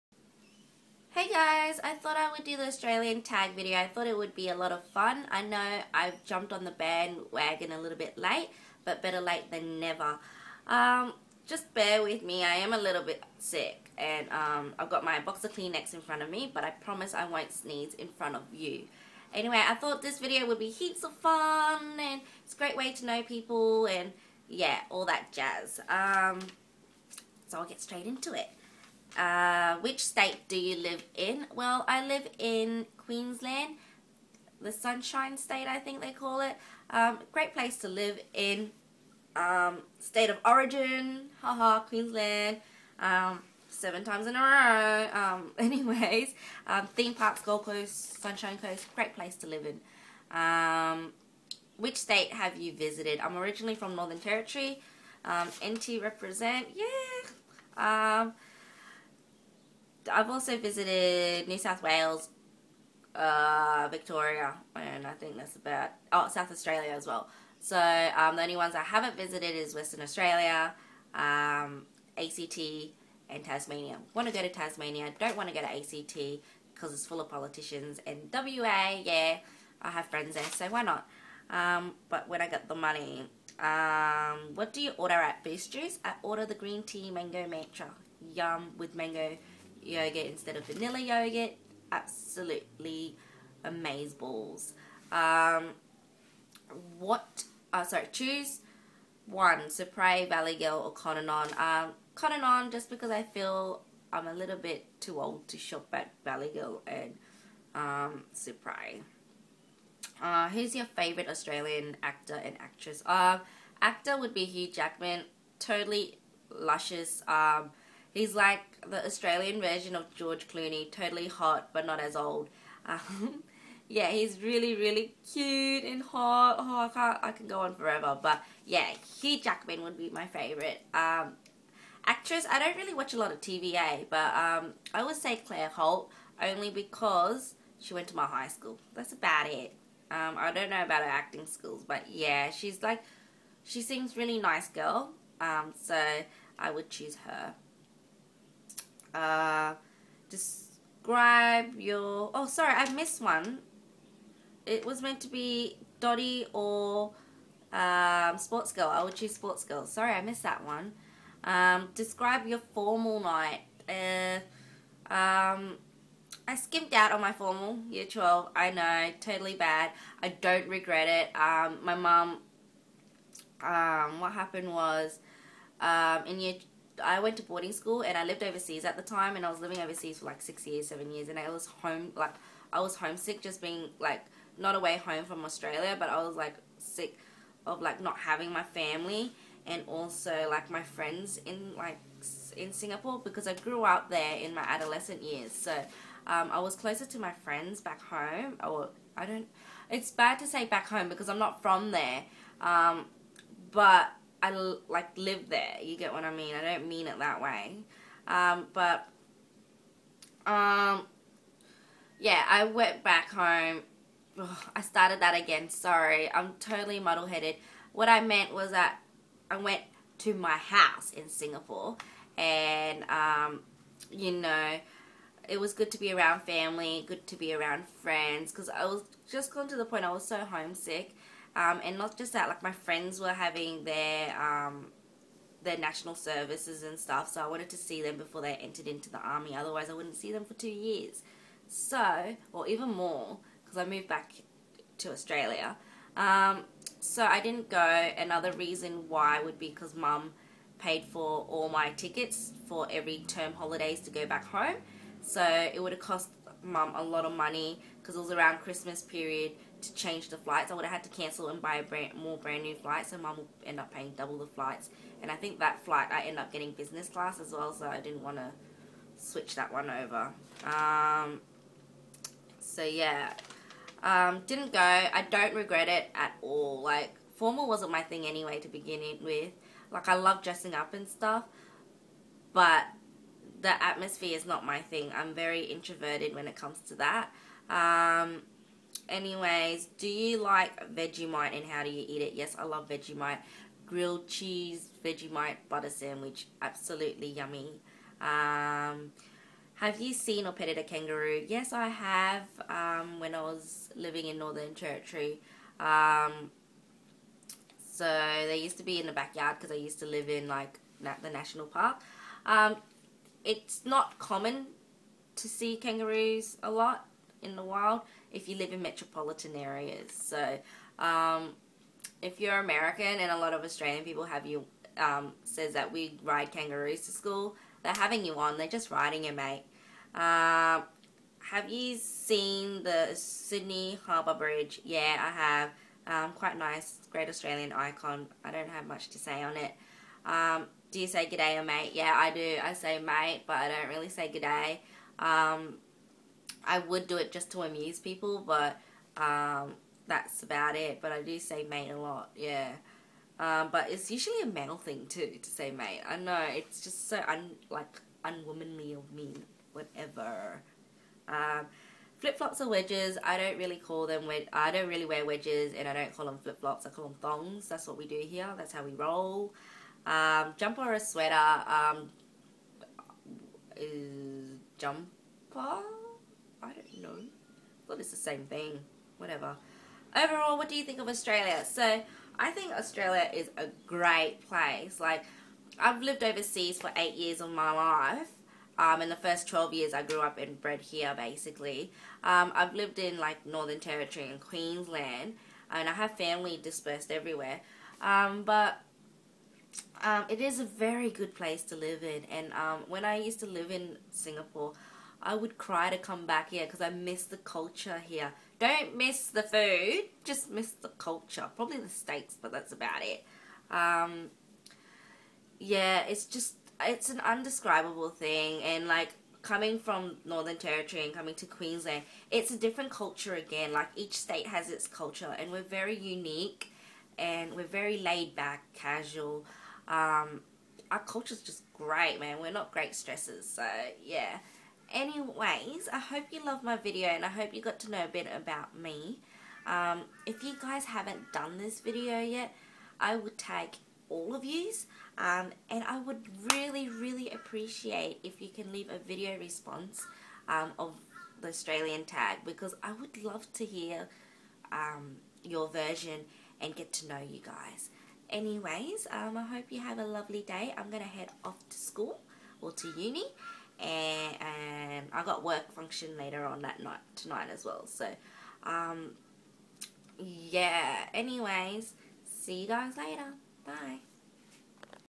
The Australian tag
Hi guys! thought this video would be heaps of fun! great way to know each other and show appreciation of Australian Youtubers and the country :D

Would love to see your videos so please leave a response.

Have a nice day xoxo